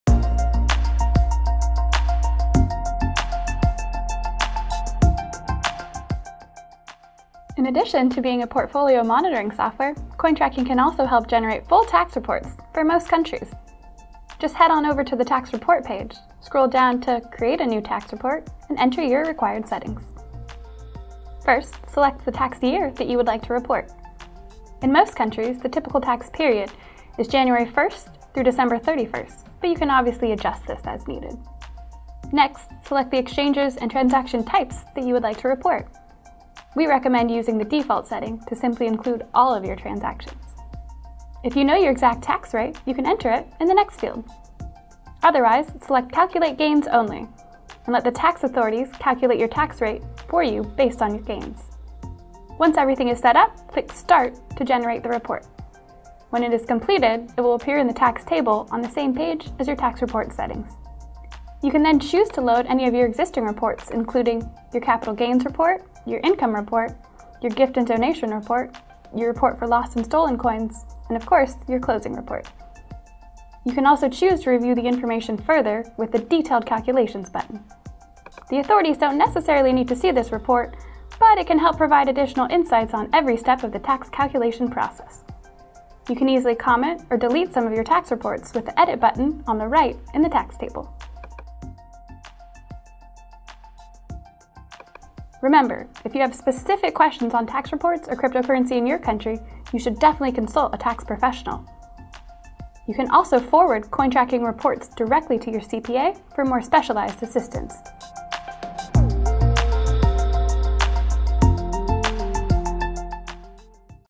CoinTracking How-To (part 15) - Creating Tax Reports
New to CoinTracking? Questions on a specific topic? Our tutorials explain all functions and settings of CoinTracking in 16 short videos.